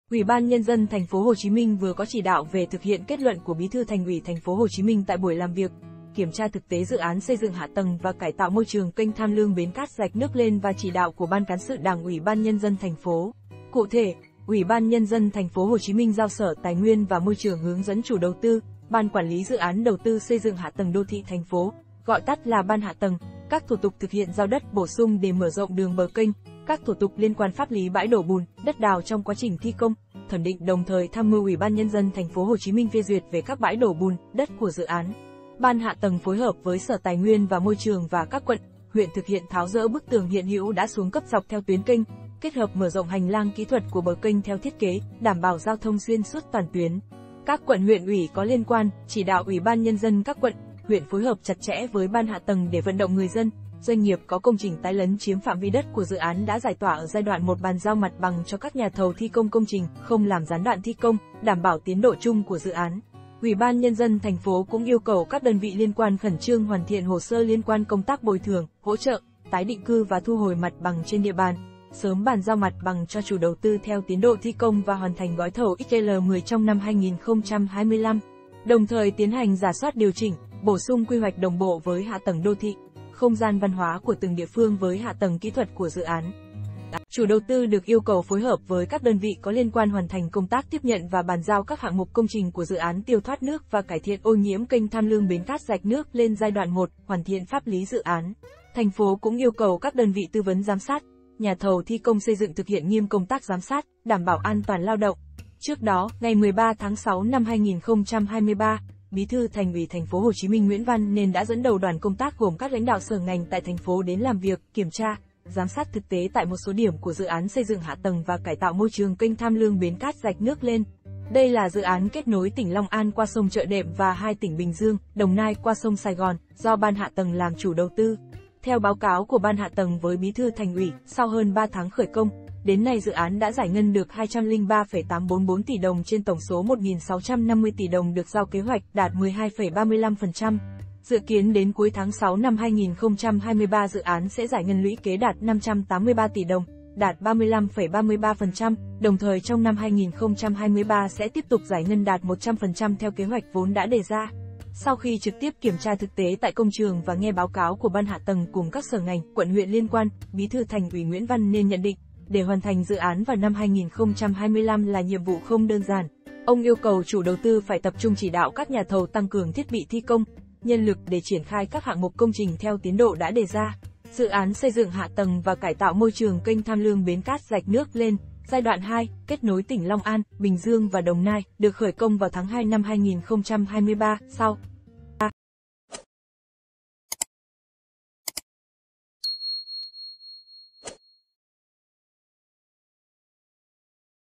THỰC HIỆN KẾT LUẬN DỰ ÁN XÂY DỰNG HẠ TẦNG KÊNH THAM LƯƠNG - BẾN CÁT - RẠCH NƯỚC LÊN
Ủy ban nhân dân  Thành phố Hồ Chí Minh vừa có chỉ đạo về thực hiện kết luận của Bí thư Thành ủy  Thành phố Hồ Chí Minh tại buổi làm việc, kiểm tra thực tế dự án xây dựng hạ tầng và cải tạo môi trường kênh Tham Lương – Bến Cát – rạch Nước Lên và chỉ đạo của Ban Cán sự đảng Uỷ ban nhân dân Thành phố. Cụ thể, Ủy ban nhân dân  Thành phố Hồ Chí Minh giao Sở Tài nguyên và Môi trường hướng dẫn chủ đầu tư (Ban quản lý Dự án Đầu tư xây dựng hạ tầng đô thị Thành phố, gọi tắt là Ban Hạ tầng) các thủ tục thực hiện giao đất bổ sung để mở rộng đường bờ kênh, các thủ tục liên quan pháp lý bãi đổ bùn, đất đào trong quá trình thi công, thẩm định đồng thời tham mưu Uỷ ban nhân dân  Thành phố Hồ Chí Minh phê duyệt về các bãi đổ bùn, đất của dự án. Ban Hạ tầng phối hợp với Sở Tài nguyên và Môi trường và các quận, huyện thực hiện tháo dỡ bức tường hiện hữu đã xuống cấp dọc theo tuyến kênh, kết hợp mở rộng hành lang kỹ thuật của bờ kênh theo thiết kế, đảm bảo giao thông xuyên suốt toàn tuyến. Các quận huyện ủy có liên quan, chỉ đạo ủy ban nhân dân các quận, huyện phối hợp chặt chẽ với Ban Hạ tầng để vận động người dân, doanh nghiệp có công trình tái lấn chiếm phạm vi đất của dự án đã giải tỏa ở giai đoạn 1 bàn giao mặt bằng cho các nhà thầu thi công công trình, không làm gián đoạn thi công, đảm bảo tiến độ chung của dự án. Ủy ban nhân dân Thành phố cũng yêu cầu các đơn vị liên quan khẩn trương hoàn thiện hồ sơ liên quan công tác bồi thường, hỗ trợ, tái định cư và thu hồi mặt bằng trên địa bàn, sớm bàn giao mặt bằng cho chủ đầu tư theo tiến độ thi công và hoàn thành gói thầu XL-10 trong năm 2025. Đồng thời tiến hành rà soát điều chỉnh, bổ sung quy hoạch đồng bộ với hạ tầng đô thị, không gian văn hóa của từng địa phương với hạ tầng kỹ thuật của dự án.